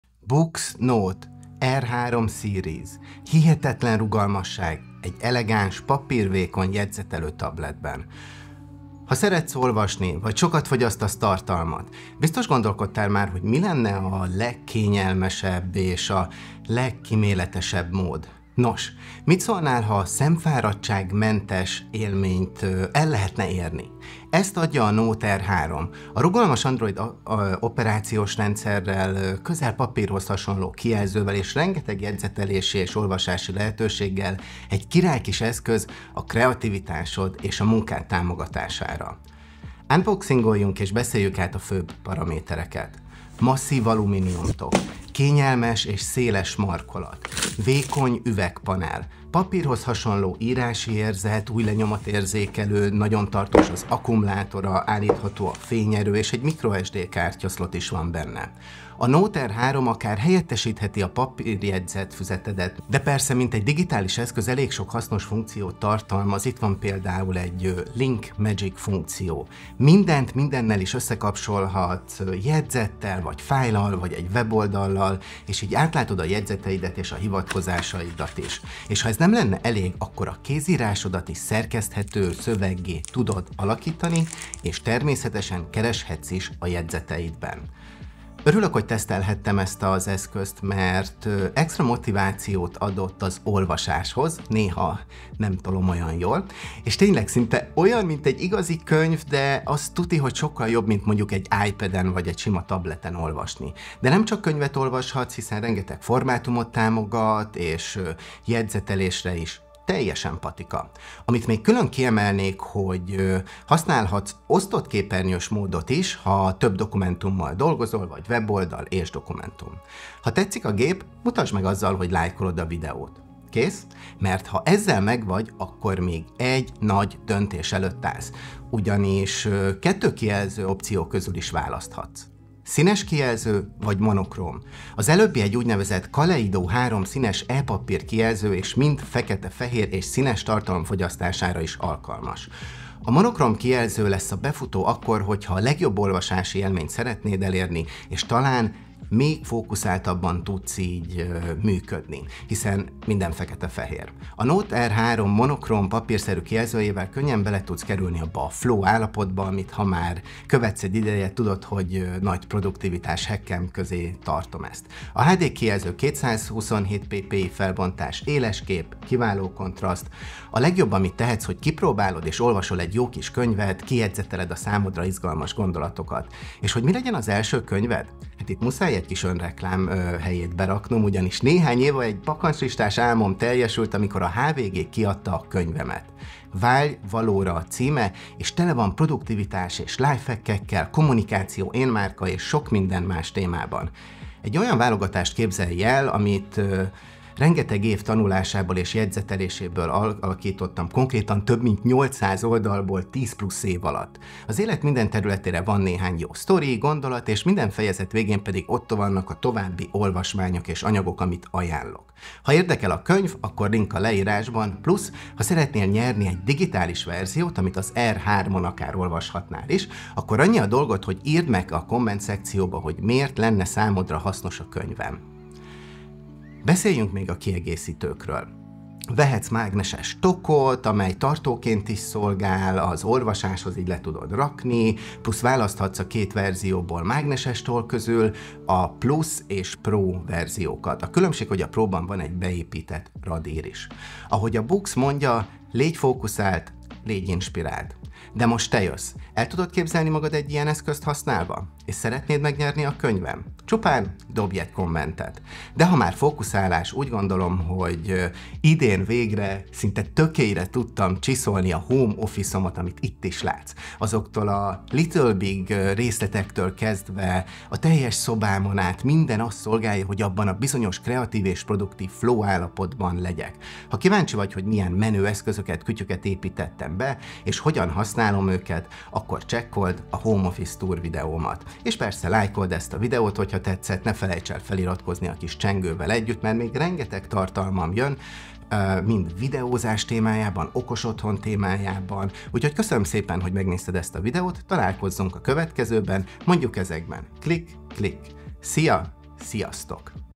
HA EBOOK OLVASÓ, EZT SZEREZD BE ❌ Boox Note Air3 Series
Az összes ide vonatkozó linket megtalálod itt a leírásban lentebb.

Ha szeretsz olvasni vagy sokat fogyasztasz tartalmat, biztos gondolkodtál már, hogy mi a legkíméletesebb mód… Nos, mit szólnál ha "szemfáradtság-mentes" élményt kaphatnál? Ezt adja a Note Air3. A rugalmas Android operációs rendszerrel, közel papírhoz hasonló kijelzővel és rengeteg jegyzetelési és olvasási lehetőséggel egy király kis eszköz a kreativitásod és munkád támogatására. De inkább nézd meg 😎.